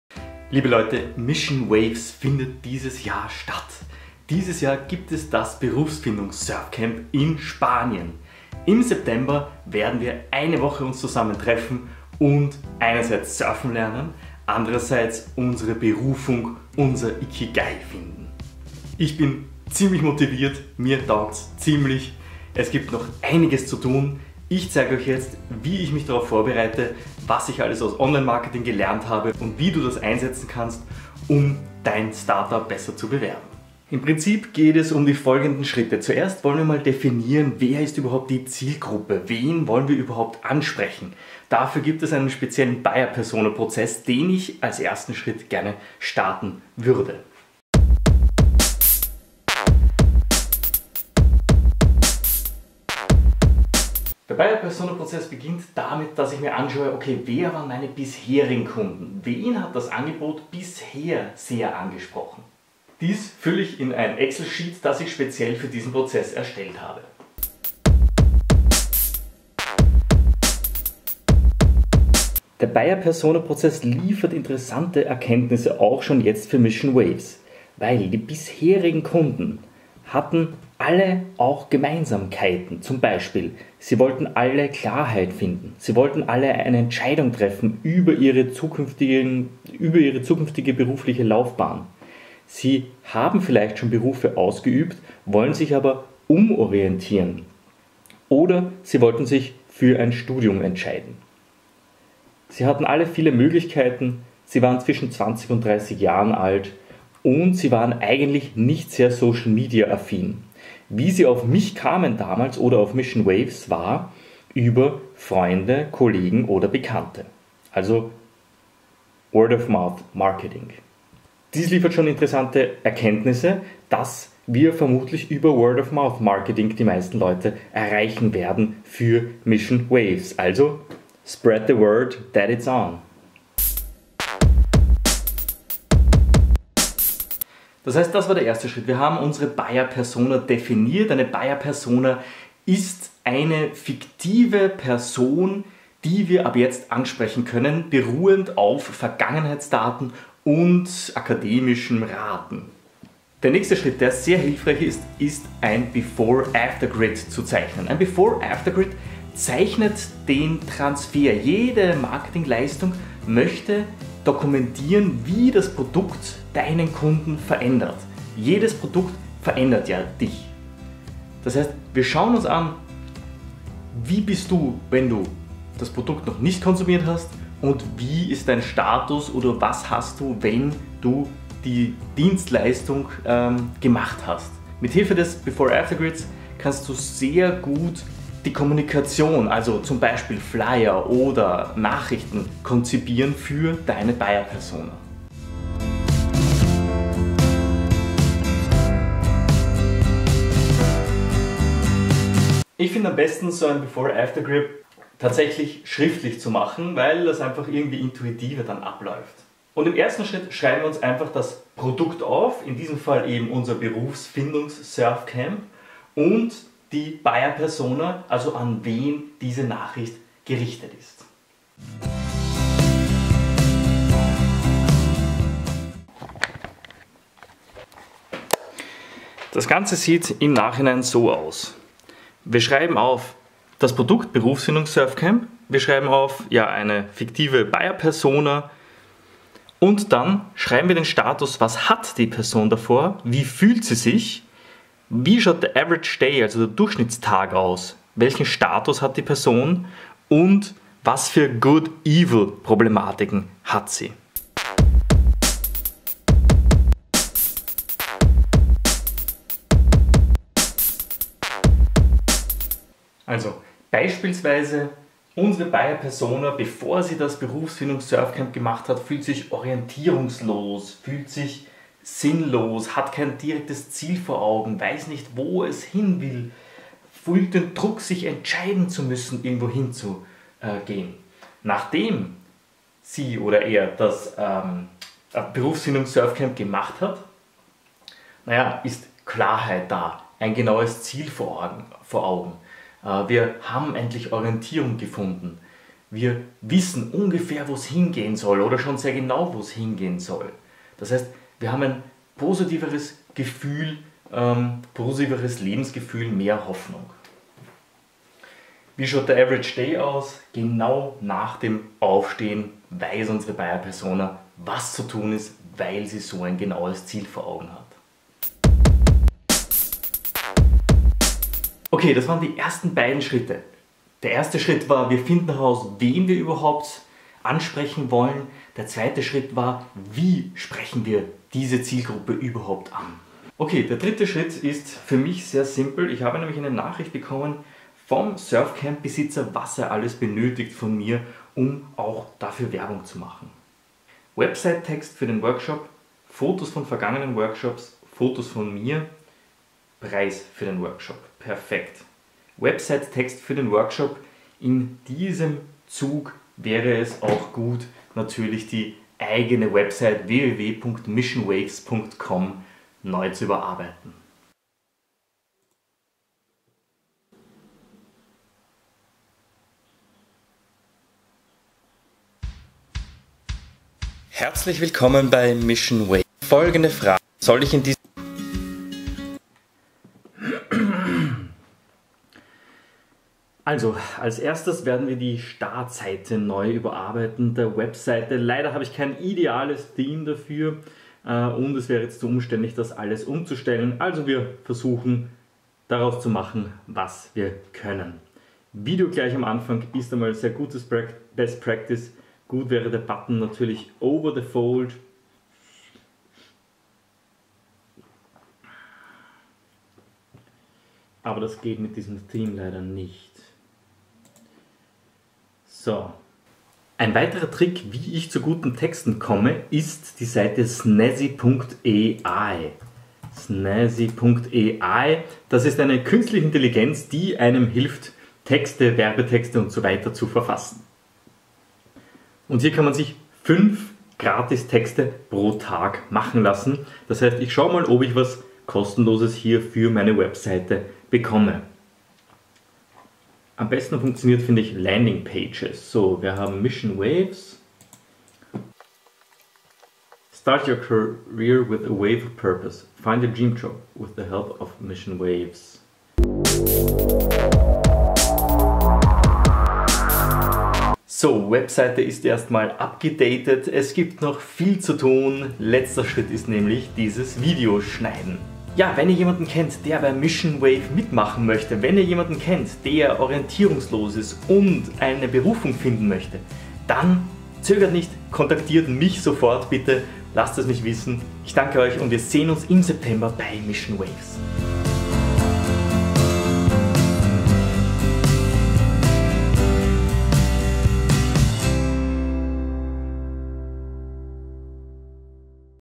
Berufsfindungssurfcamp startet wieder!
Das Berufsfindungs-Surfcamp findet dieses Jahr statt! In diesem Video besprechen wir, wie ich Marketing dafür starte!

The Seabird Vlog 560